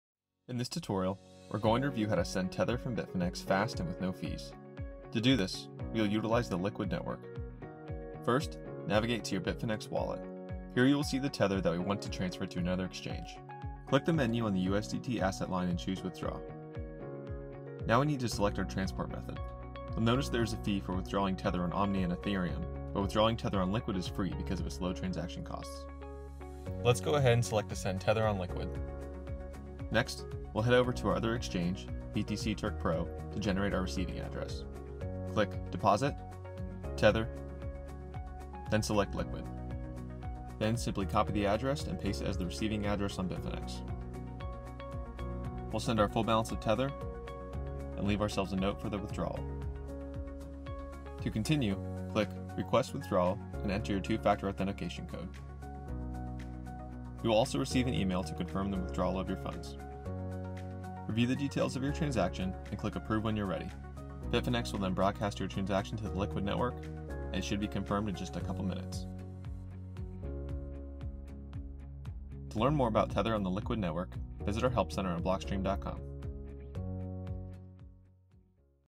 Liquid Network | How to send Tether from Bitfinex using Liquid
A quick guide for utilizing the Liquid Network to send Tether from Bitfinex, fast and with no fees.

The Liquid sidechain represents a major upgrade over Tether’s original platform, the Omni Layer, offering faster settlements, Confidential Transactions, and sophisticated multisig security.